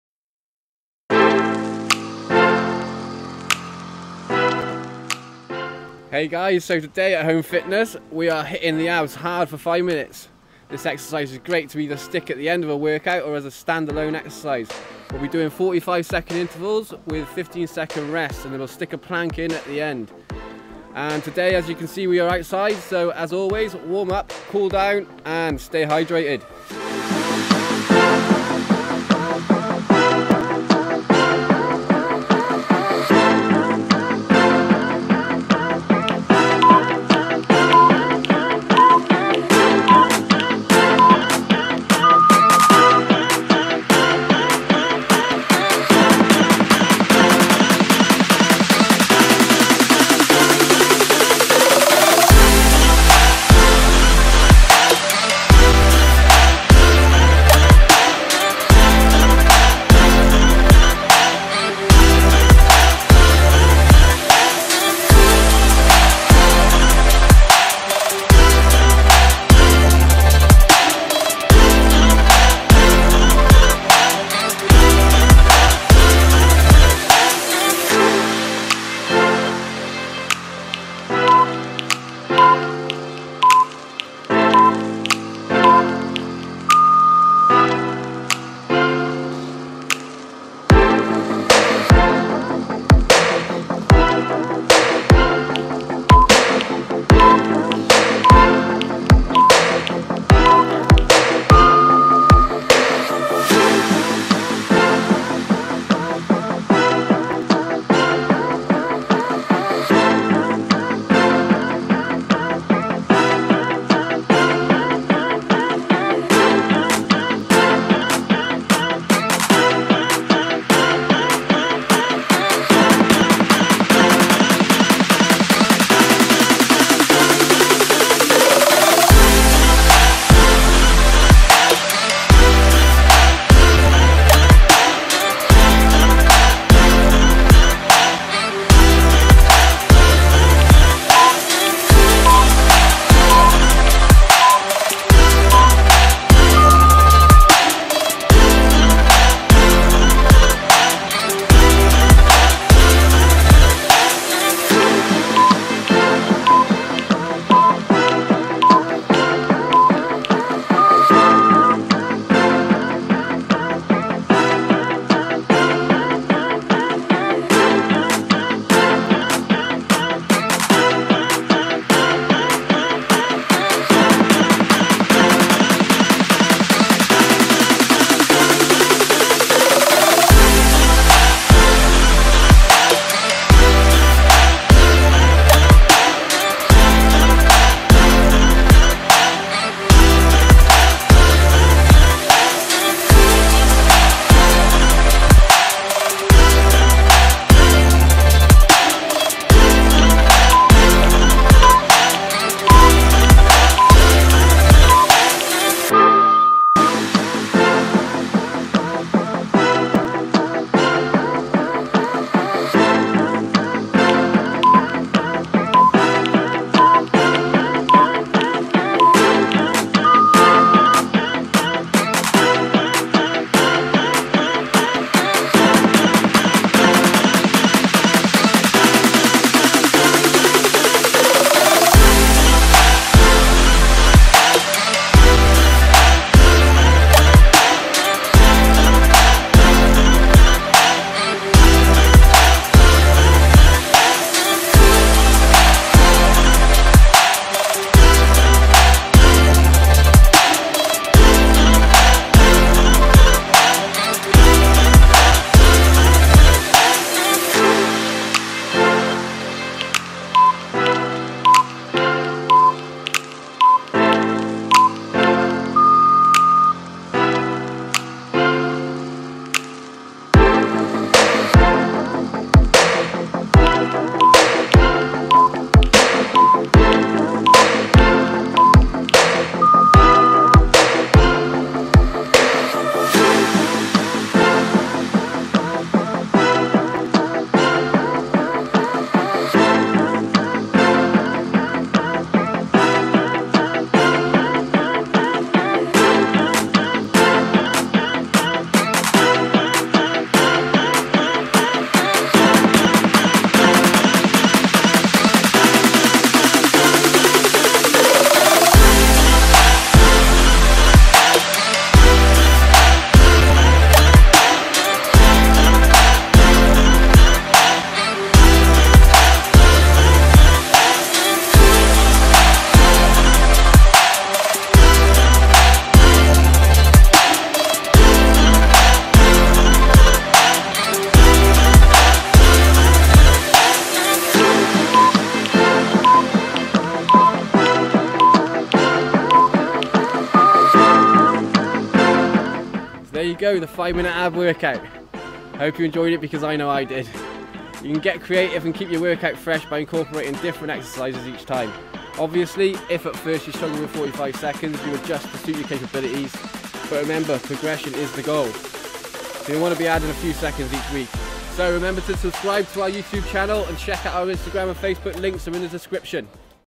5 Minute Interactive Home Ab Workout - Home Fitness
here we have our 5 Minute Interactive Home Ab Workout, designed for you to preform along side the video! lets stay fit through this crisis! we can do this!